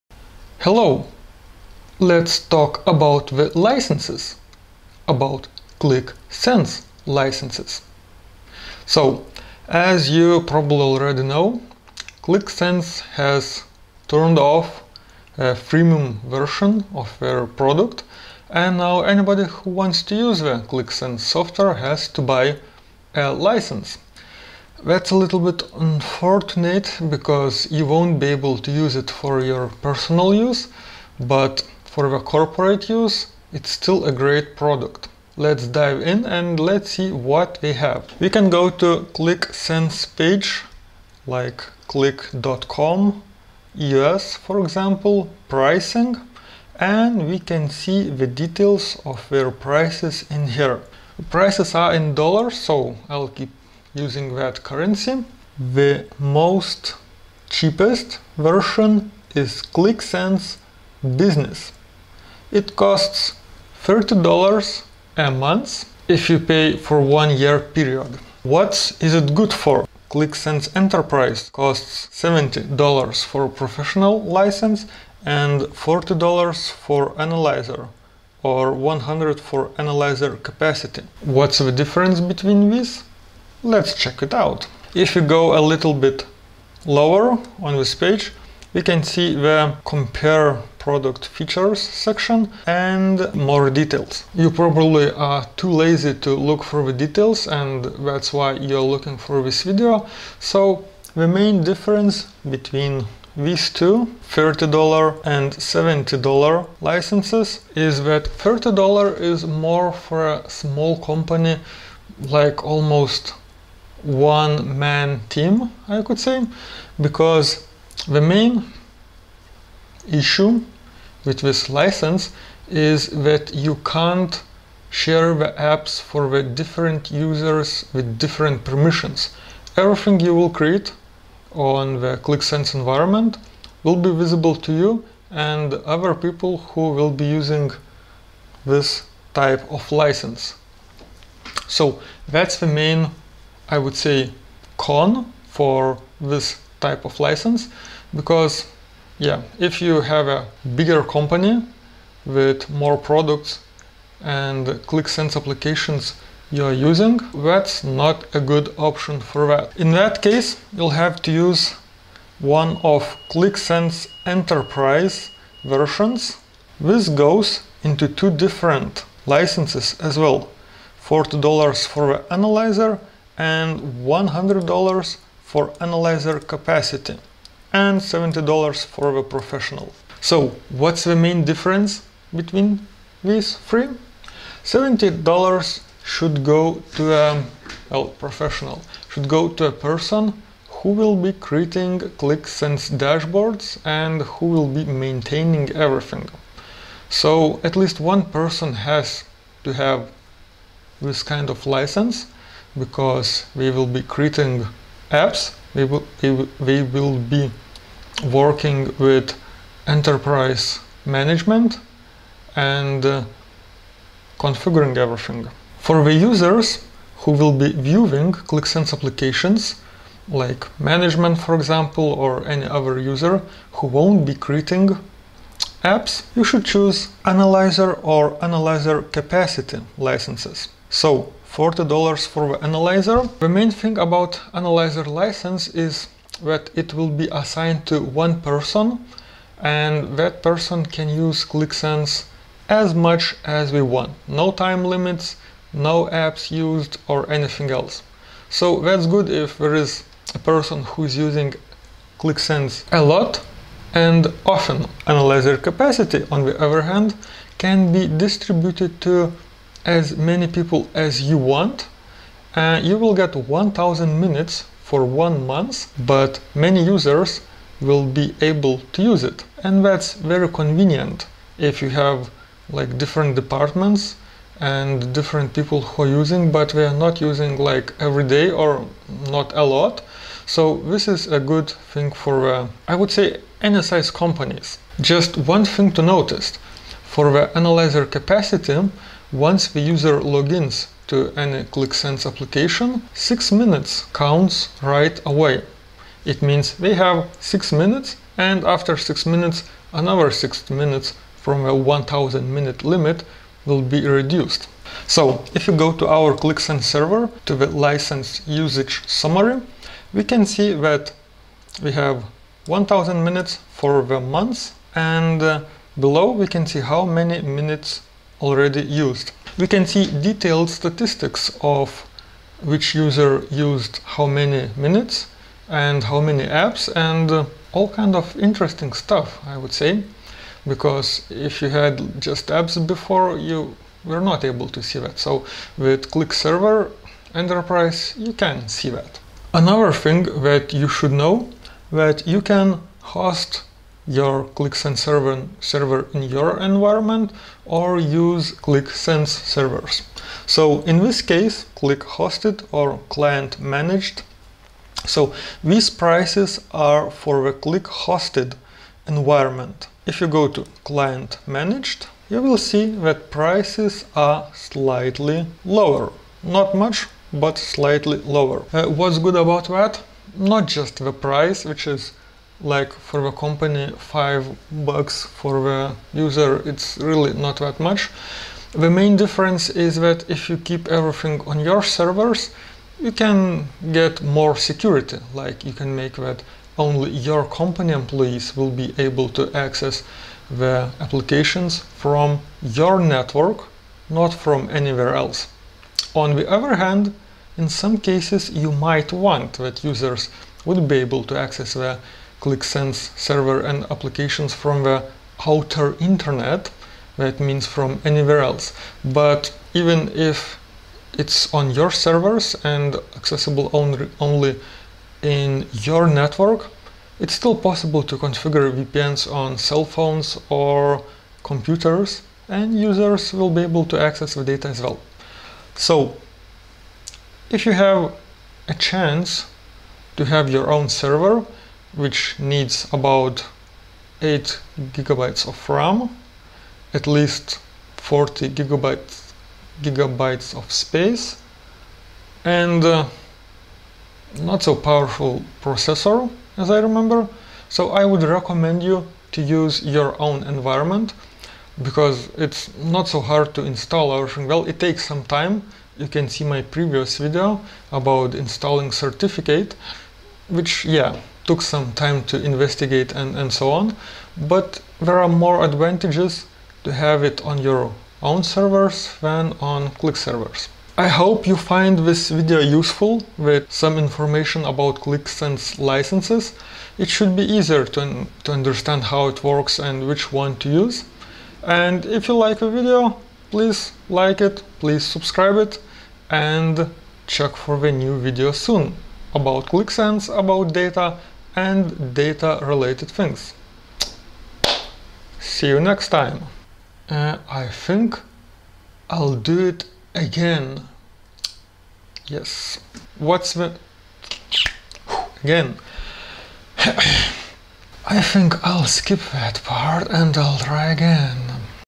Qlik Sense pricing and licenses explained!
Many prices, many types of licenses, a lot of information does not help to decide what to choose. Well, not anymore, everything about Qlik Sense licenses and pricing is explained in this video.

00:00 Why do you need a Qlik Sense license?
00:37 Checking license information on Qlik website
01:21 Difference between the licenses
01:47 Information about Qlik Sense Business license
02:39 Information about Qlik Sense Enterprise SaaS licenses
03:04 Qlik Sense Enterprise SaaS Professional license explained
03:35 Qlik Sense Enterprise SaaS Analyzer license explained
04:14 Qlik Sense Enterprise SaaS Analyzer Capacity license explained
05:39 Differences between "Qlik hosted" and "Client managed" license types
07:50 Thank you for watching!
08:16 FAILS

Gear I use:
External HDD I recomend